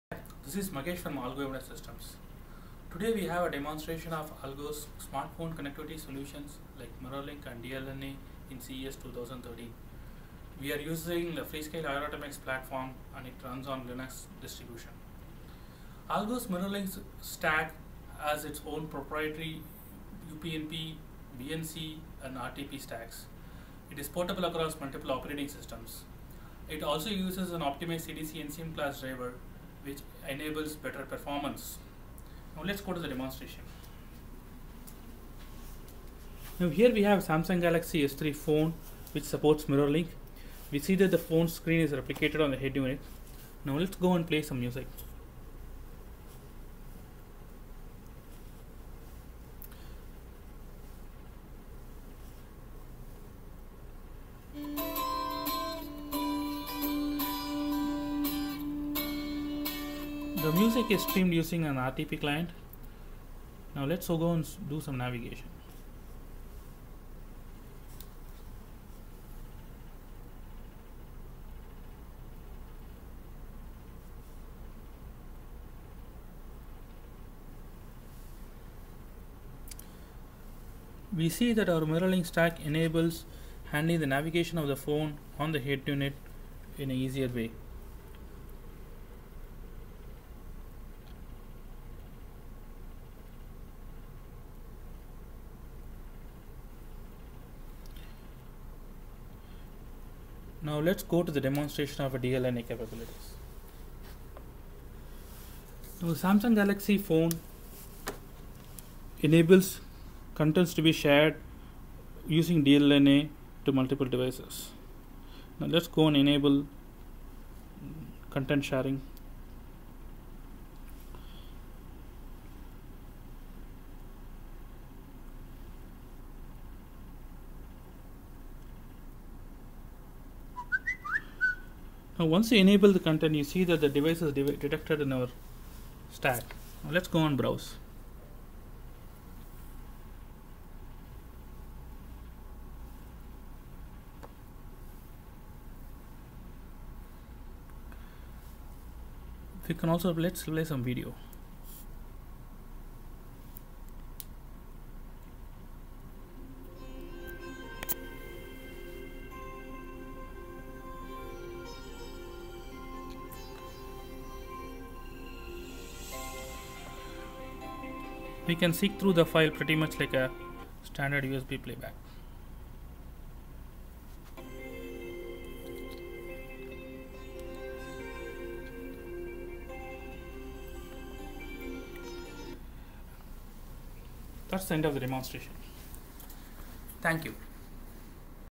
AllGo CES 2013 MirrorLink & DLNA Demonstration
In this Video, AllGo demonstrates its MirrorLink Client SDK and DLNA Stack in CES 2013, which is portable across multiple operating systems. The test device used is Samsung Galaxy S3.